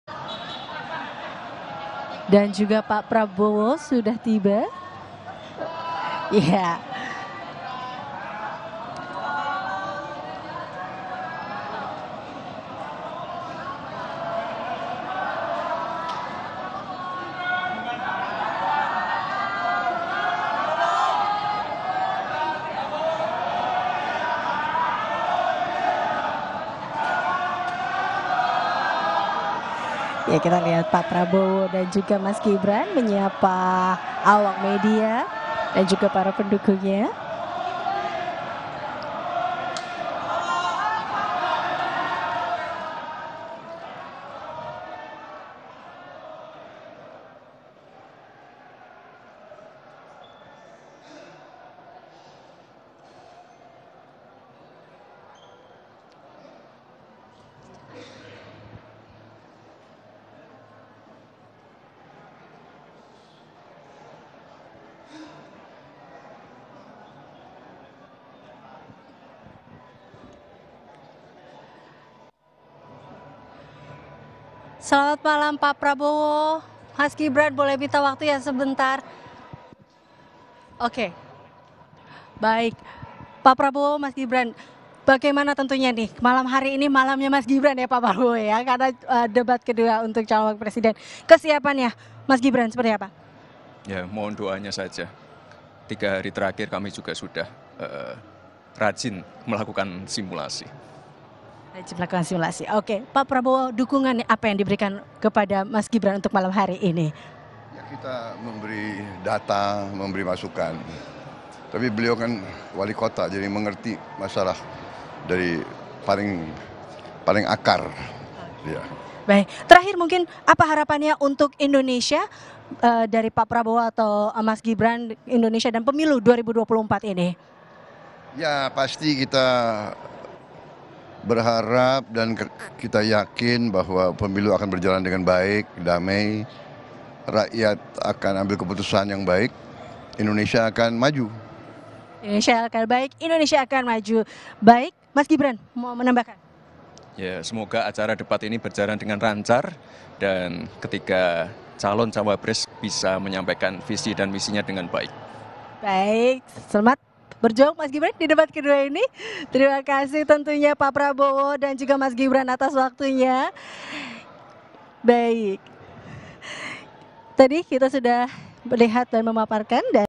Detik-Detik Prabowo-Gibran Tiba di Arena Debat Cawapres Bahas Persiapan Materi
JAKARTA, KOMPASTV - Cawapres nomor urut 2 Gibran Rakabuming tiba di arena Debat Cawapres, JCC, Jakarta, Jumat (22/12). Gibran tiba didampingi Capres Prabowo Subianto.

Debat Cawapres untuk Pilpres 2024 digelar di JCC, Jakarta, Jumat (22/12).

Ketiga cawapres, Muhaimin Iskandar atau Cak Imin, Gibran Rakabuming Raka, dan Mahfud MD beradu gagasan terkait tema yang telah ditentukan.

Dalam debat cawapres kali ini, tema yang dibahas antara lain meliputi ekonomi, infrastruktur, hingga perkotaan.

Ini menjadi debat Pilpres 2024 kedua setelah sebelumnya tiga capres Anies Baswedan, Prabowo Subianto, dan Ganjar Pranowo jalani debat perdana.